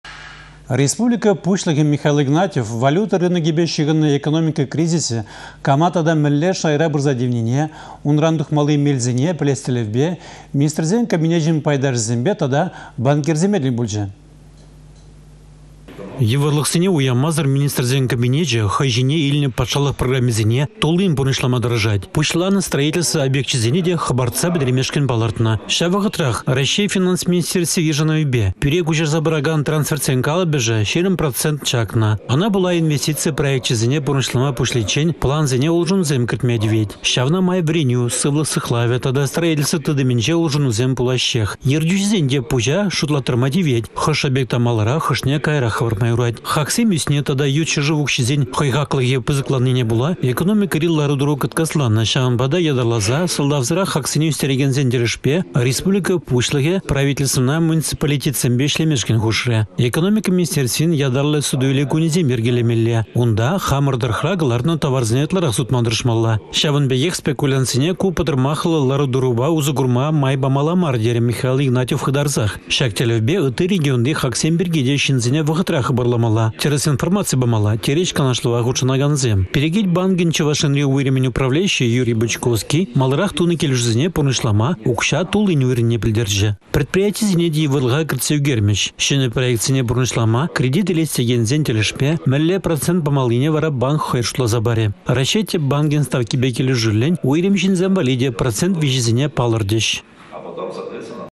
Кризисран тухма пулать-и?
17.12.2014
Республика Пуçлăхĕ Михаил Игнатьев валюта рынокĕпе çыхăннă экономика кризисĕ кама тата мĕнле шайра пырса тивнине, унран тухмалли мелсене пĕлес тĕллевпе Министрсен Кабинечĕн пайташĕсемпе тата банкирсемпе тĕл пулчĕ.